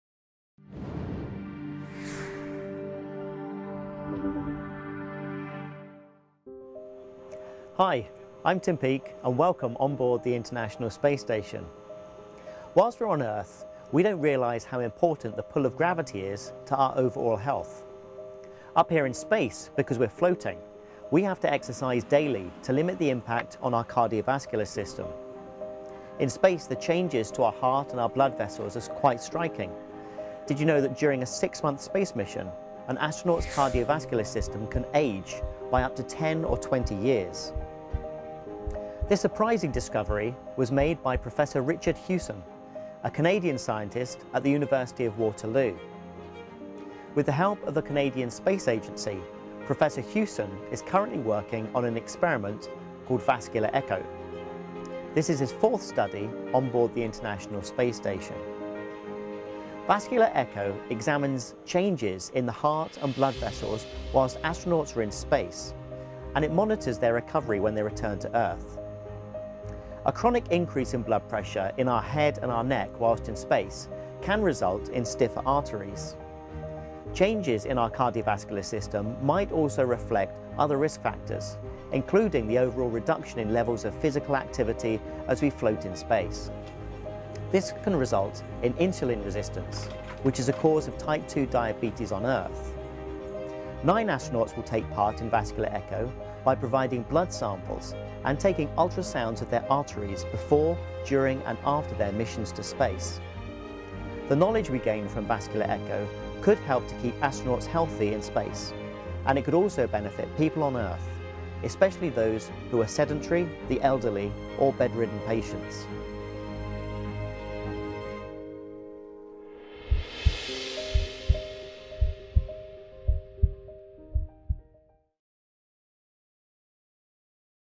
Using Space to Fight Cardiovascular Disease with Astronaut Tim Peake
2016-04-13 - Did you know that during a six-month space mission, an astronaut’s cardiovascular system can age by up to 10 or 20 years? ESA astronaut Tim Peake will take part in the Canadian experiment Vascular Echo aboard the ISS that will examine changes in heart and blood vessels of astronauts in space. (Credits: Canadian Space Agency, NASA)